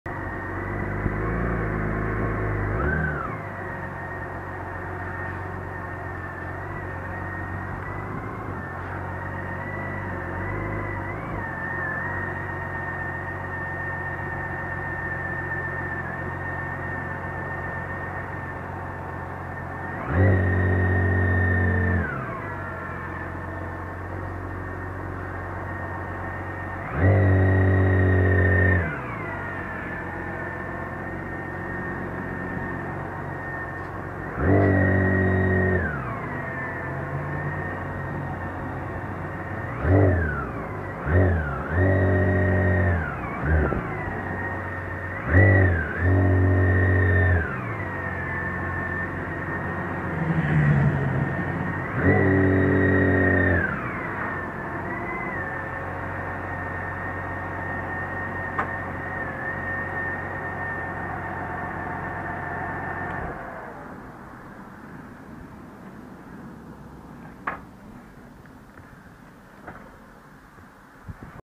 Tutoriel marche arrière BMW K1600 GT 2018
Tutoriel d'une marche arrière avec la K1600GT 2018, c'est une option au catalogue. Mon avis: Je trouve que ce n'est pas assez progressif, en appuyant sur le bouton du démarreur, c'est ON ou OFF. Mais au cas d'un stationnement dans le mauvais sens, ça peut être très utile pour sortir la moto de la pente à reculons.